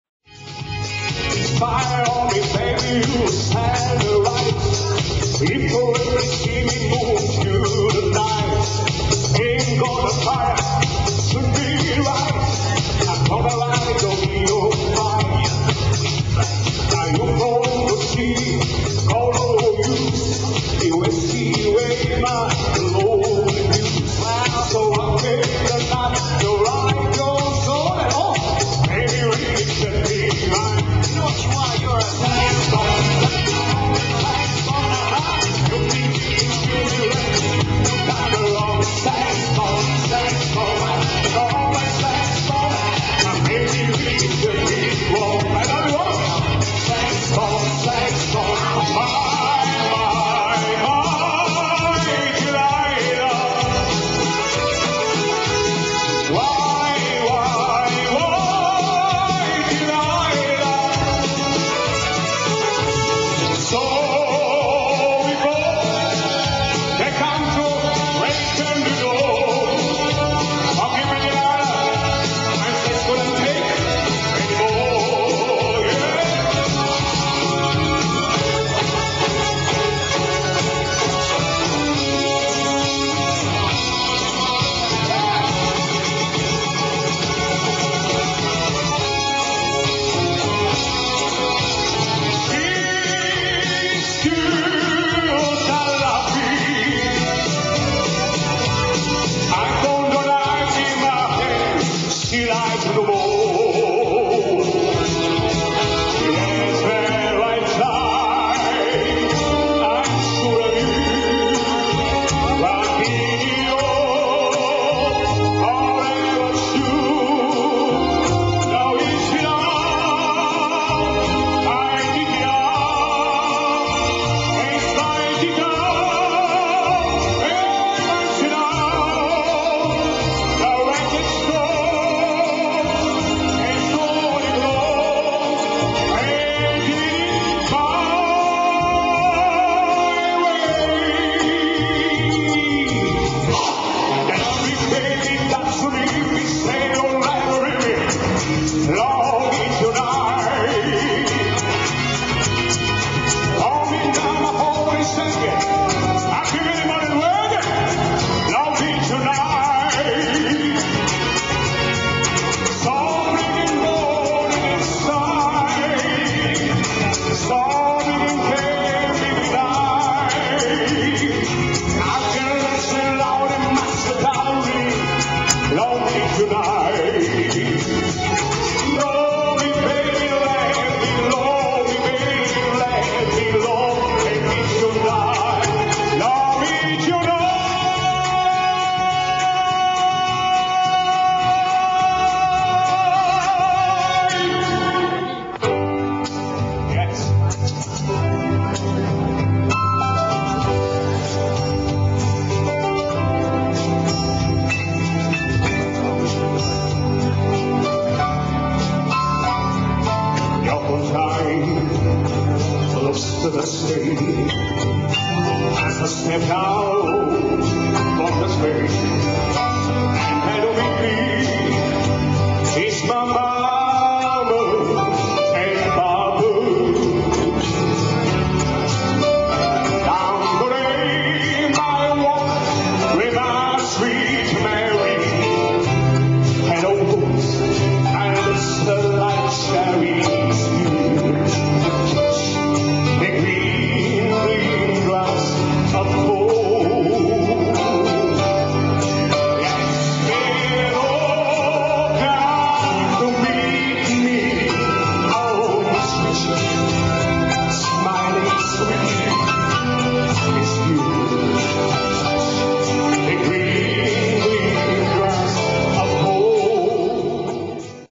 Tom Jones sons sing Mitko Frenski - live show
Songs from Tom Jones singing live from the Bulgaria star singer Mitko Frenski